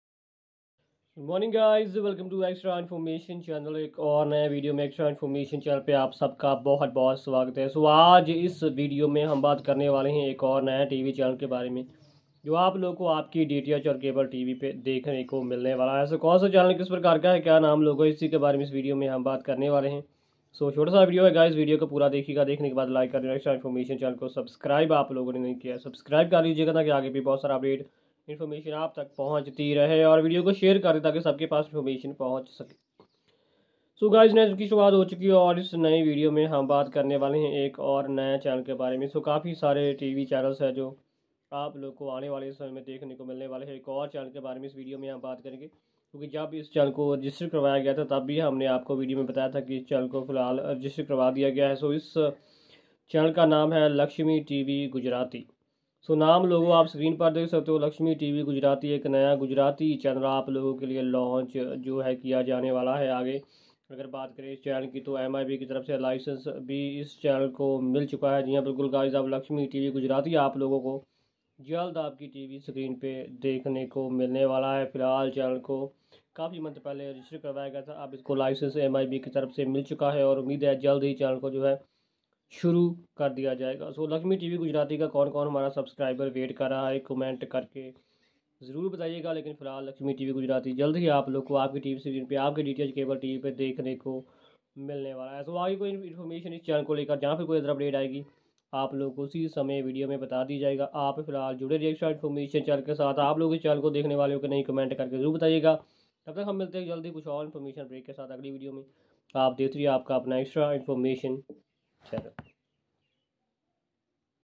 LAXMI TV NEW CHANNEL to be Launched On Tata Play Airtel Digital TV Dish TV & DD Free Dish || 20 Feb
LAXMI TV NEW CHANNEL to be Launched On Tata Play Airtel Digital TV Dish TV & DD Free Dish DTH || 20 February 2024

hello good morning

Laxmi TV Gujarati
On Tata Play
Airtel DTH
dish TV
Coming soon

Channel host:
Gurbaj Singh

Note : 🙏

enjoy Tuesday